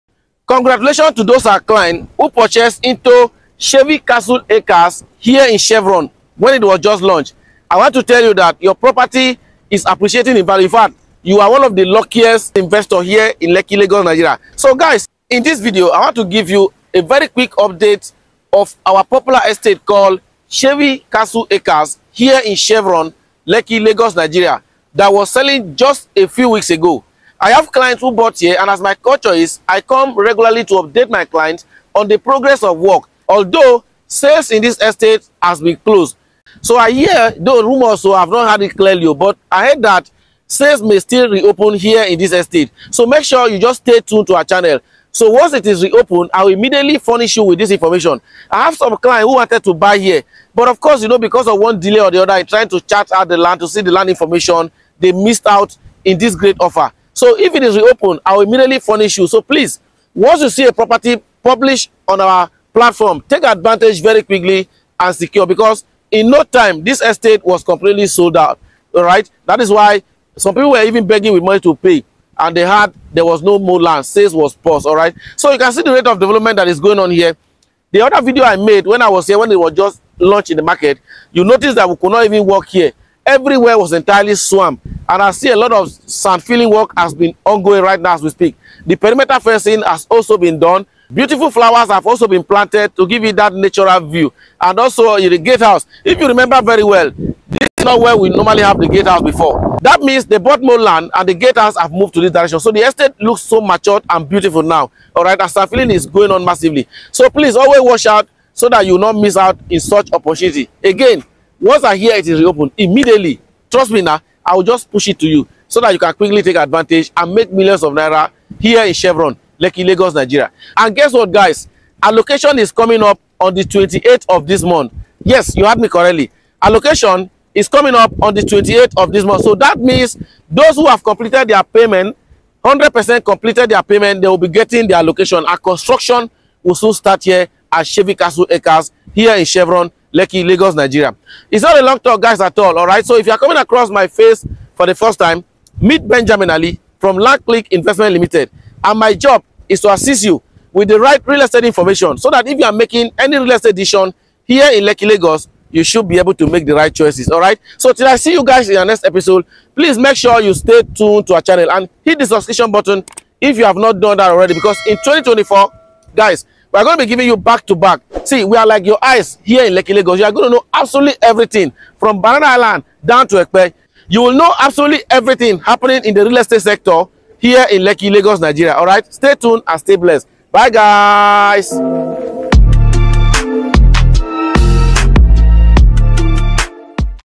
CHEVY CASTLE ACRES Allocation Day | Lekki Lagos Nigeria
This is the most Affordable and best Real Estate investment on the lekki Lagos corridor.

Chevy Castle Acres is located on the Chevron Drive Lekki Lagos Nigeria and promises good return on your investment in next few weeks

Landclickng.com